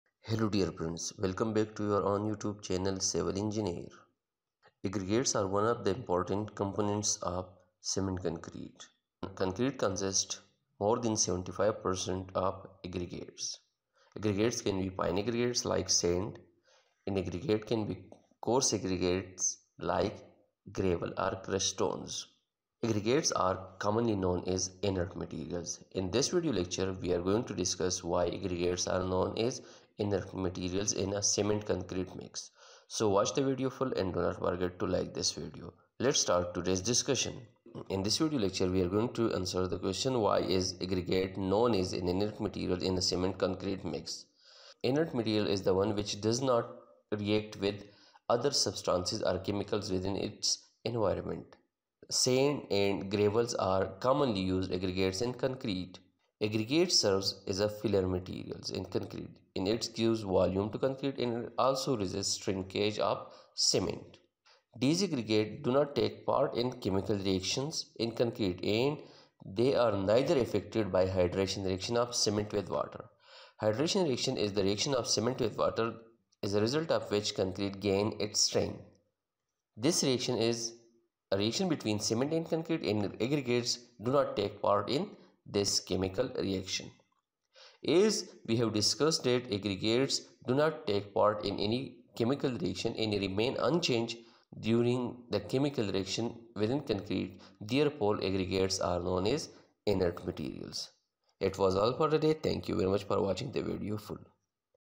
Why AGGREGATES are known as Inert Materials in a Cement Concrete Mix?
In this short video lecture, we are going to discuss, Why AGGREGATES are known as Inert Materials in a Cement Concrete Mix? | Inert Materials in Concrete
Aggregates are important components of Cement Concrete.
Fine Aggregates
Coarse Aggregates
Inert Material is the one that does not react with other substances within its environment.
Sand (Fine Aggregates) and Gravel (Coarse Aggregates) are commonly used aggregates in concrete.
Aggregates serve as a filler material in concrete and give volume to concrete.
Aggregates do not take part in chemical reactions in concrete and are neither affected by the hydration of cement with water.
As aggregates do not take any part in a chemical reaction and remain unchanged, therefore aggregates are known as INERT Materials.